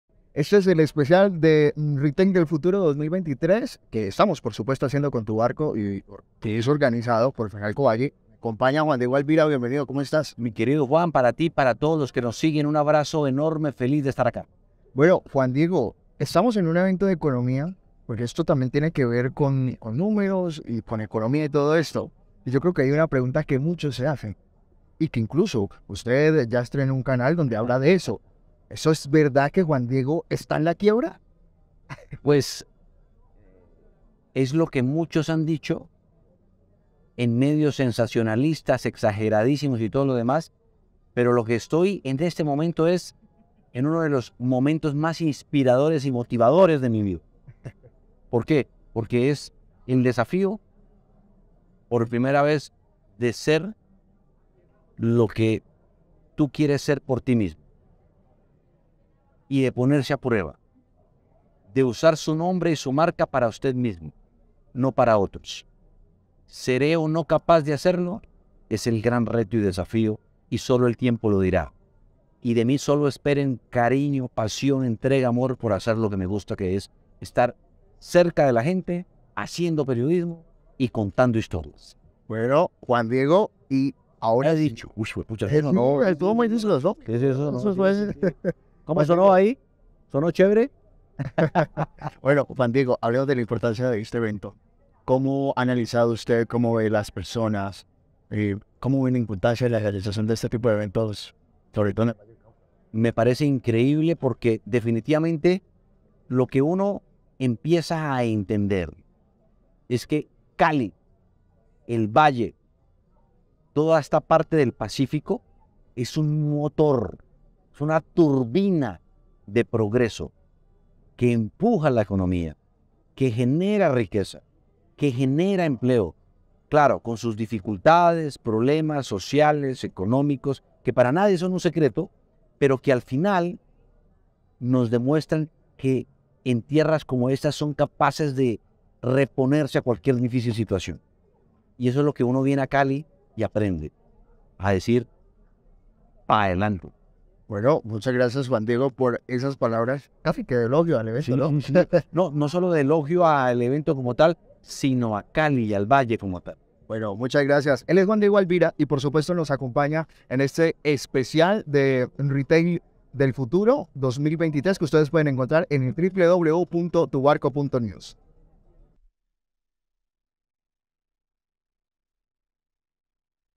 Juan Diego Alvira: "Estoy en uno de los momentos más inspiradores de mi vida", durante el retail
El periodista @juandiegoalviraperiodista estuvo en el Retail del Futuro 2023 como presentador. Y habló de su vida en el momento, y cómo los mensajes de empresarios sobre sus experiencias de cambio, también le dejan un mensaje importante sobre la transformación para buscar  oportunidades.

"Estoy en uno de los momentos más inspiradores"; así mismo, desmiente a algunos medios que han dicho que se encuentra "en la quiebra".

De su visión sobre el Valle del Cauca, señala que tiene una fuerza empresarial que impulsa el desarrollo del suroccidente colombiano y del país, lo cual ha podido evidenciar en su participación en el retaildelfuturo2023